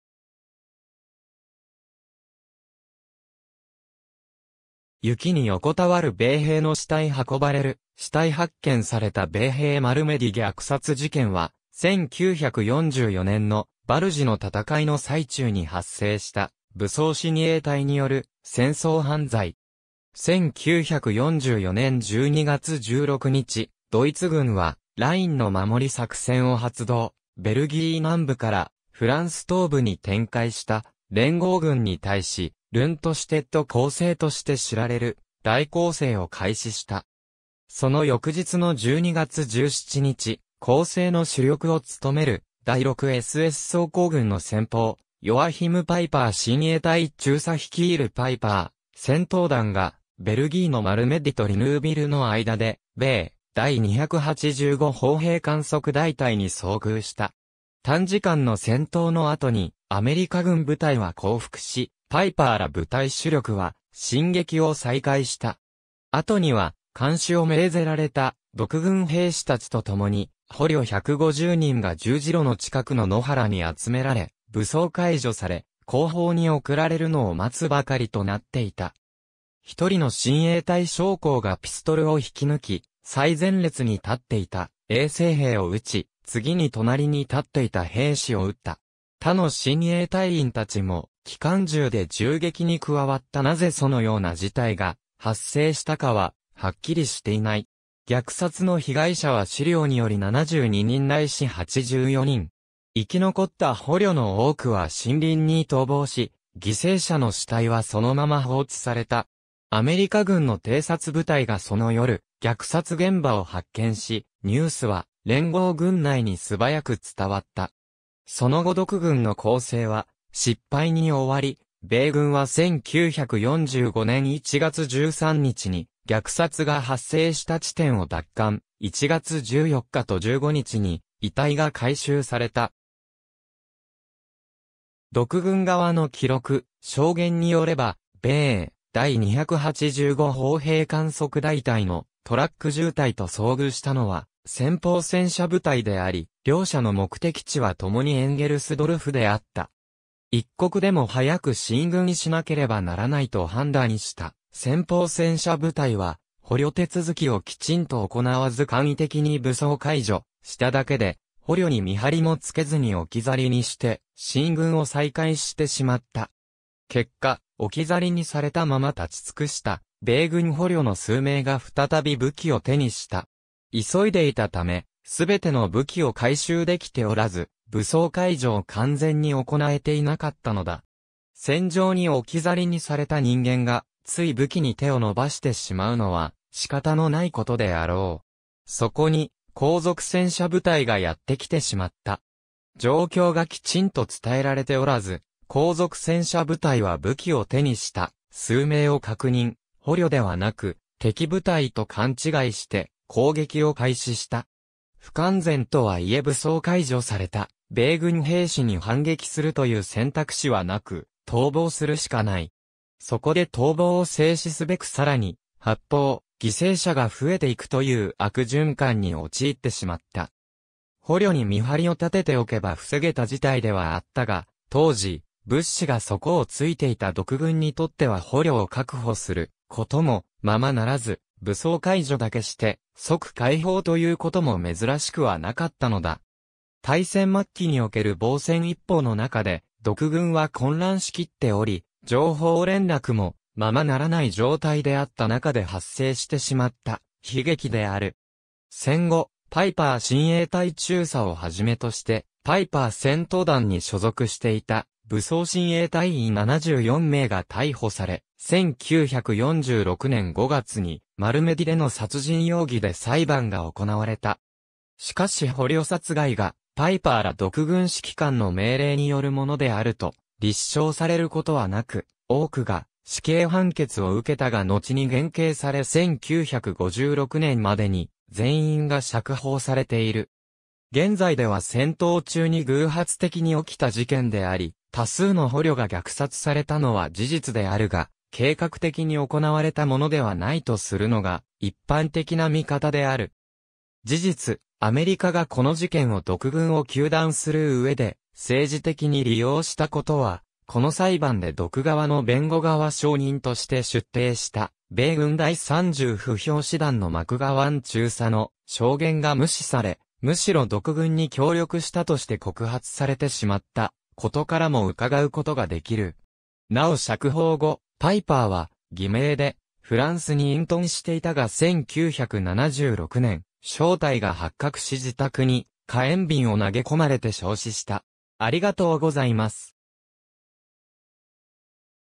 マルメディ虐殺事件
ナチス・ドイツの戦争犯罪
雪に横たわる米兵の死体 運ばれる死体 発見された米兵 マルメディ虐殺事件（マルメディぎゃくさつじけん、Malmédy massacre）は、1944年のバルジの戦いの最中に発生した武装親衛隊による戦争犯罪。
1944年12月16日、ドイツ軍は「ラインの守り」作戦を発動、ベルギー南部からフランス東部に展開した連合軍に対し、ルントシュテット攻勢（連合国側の呼称「バルジの戦い」）として知られる大攻勢を開始した。
その翌日の12月17日、攻勢の主力を務める第6SS装甲軍の先鋒、ヨアヒム・パイパー親衛隊中佐率いるパイパー戦闘団がベルギーのマルメディとリヌーヴィルの間で米第285砲兵観測大隊に遭遇した。
短時間の戦闘の後にアメリカ軍部隊は降伏し、パイパーら部隊主力は進撃を再開した。
あとには監視を命ぜられた独軍兵士たちと共に捕虜150人が十字路の近くの野原に集められ、武装解除され後方に送られるのを待つばかりとなっていた。
「一人の親衛隊将校がピストルを引き抜き、最前列に立っていた衛生兵を撃ち、次に隣に立っていた兵士を撃った。他の親衛隊員達も機関銃で銃撃に加わった」 なぜそのような事態が発生したかははっきりしていない。
虐殺の被害者は資料により72人ないし84人。
生き残った捕虜の多くは森林に逃亡し、犠牲者の死体はそのまま放置された。
アメリカ軍の偵察部隊がその夜虐殺現場を発見し、ニュースは連合軍内に素早く伝わった。
その後独軍の攻勢は失敗に終わり、米軍は1945年1月13日に虐殺が発生した地点を奪還、1月14日と15日に遺体が回収された。
独軍側の記録、証言によれば米第285砲兵観測大隊のトラック縦隊と遭遇したのは先鋒戦車部隊であり、両者の目的地は共にエンゲルスドルフであった。
一刻でも早く進軍しなければならないと判断した先鋒戦車部隊は、捕虜手続きをきちんと行わず簡易的に武装解除しただけで、捕虜に見張りも付けずに置き去りにして進軍を再開してしまった。
結果、置き去りにされたまま立ち尽くした米軍捕虜の数名が再び武器を手にした。
急いでいた為、全ての武器を回収できておらず、武装解除を完全に行えていなかったのだ。
戦場に置き去りにされた人間が、つい武器に手を伸ばしてしまうのは仕方のない事であろう。
そこに後続戦車部隊がやって来てしまった。
状況がきちんと伝えられておらず、後続戦車部隊は武器を手にした数名を確認、捕虜ではなく敵部隊と勘違いして攻撃を開始した。
不完全とはいえ武装解除された米軍兵士に反撃するという選択肢は無く、逃亡するしかない。
そこで逃亡を制止すべく更に発砲、犠牲者が増えていくという悪循環に陥ってしまった。
捕虜に見張りを立てておけば防げた事態ではあったが、当時、物資が底をついていた独軍にとっては捕虜を確保する事もままならず、武装解除だけして即解放という事も珍しくはなかったのだ。
大戦末期における防戦一方の中で独軍は混乱しきっており、情報連絡もままならない状態であった中で発生してしまった悲劇である。
戦後、パイパー親衛隊中佐を始めとして、パイパー戦闘団に所属していた武装親衛隊員74名が逮捕され、1946年5月にマルメディでの殺人容疑で裁判が行われた。
しかし捕虜殺害がパイパーら独軍指揮官の命令によるものであると立証されることはなく、多くが死刑判決を受けたがのちに減刑され1956年までに全員が釈放されている。
現在では戦闘中に偶発的に起きた事件であり、多数の捕虜が虐殺されたのは事実であるが計画的に行われたものではないとするのが一般的な見方である。
事実、アメリカがこの事件を独軍を糾弾する上で政治的に利用した事は、この裁判で独側の弁護側証人として出廷した米軍第30歩兵師団のマクガワン中佐の証言が無視され、むしろ独軍に協力したとして告発されてしまった事からもうかがう事ができる。
なお釈放後、パイパーは偽名でフランスに隠遁していたが1976年、正体が発覚し自宅に火炎瓶を投げ込まれて焼死した。